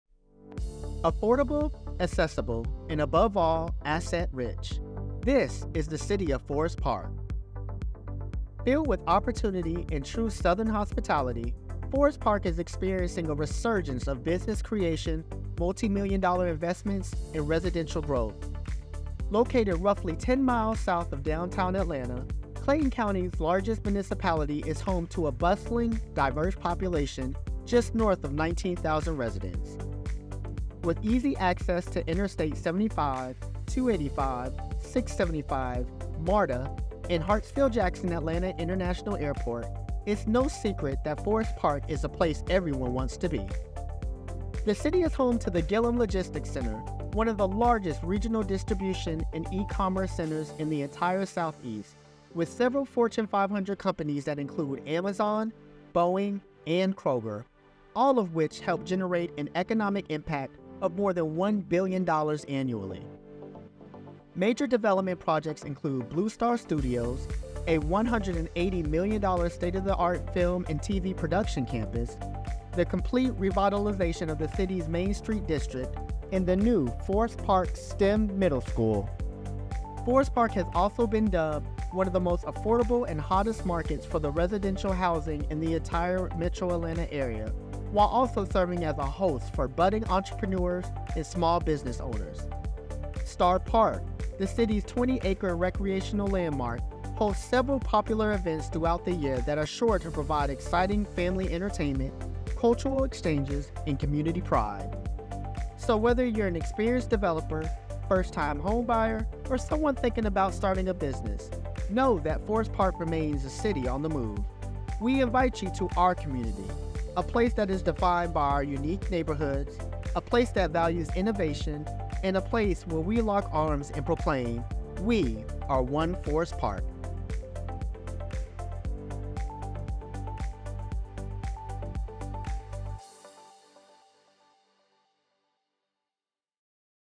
We Are OneForestPark
Filled with opportunity and true Southern hospitality, Forest Park is experiencing a resurgence of business creation, multi-million dollar investments and residential growth.